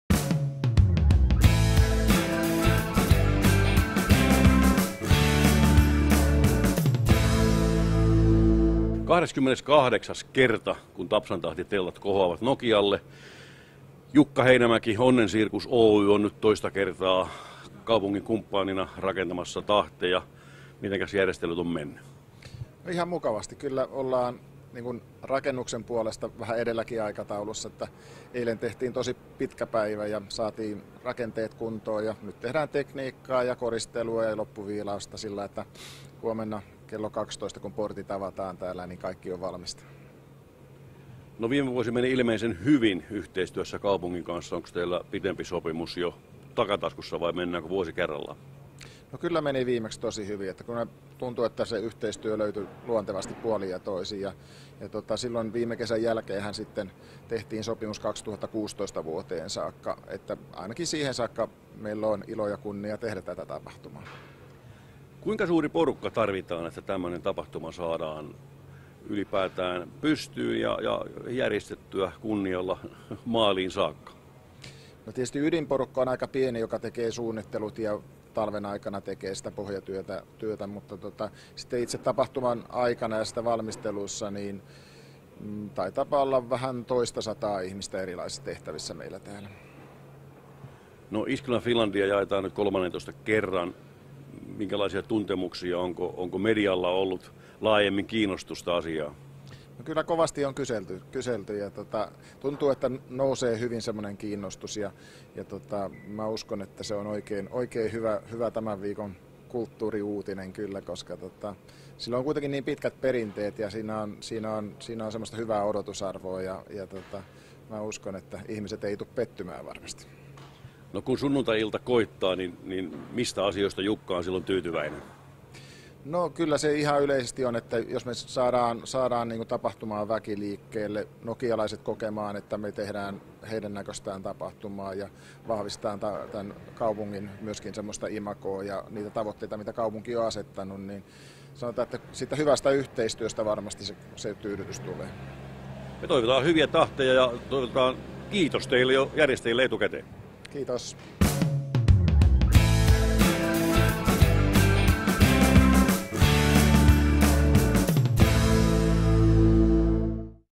Tapsan Tahdit Jukka Heinämäki mpeg4
Jukka Heinämäki kertoo järjestelyistä Tapsan Tahtien aattona.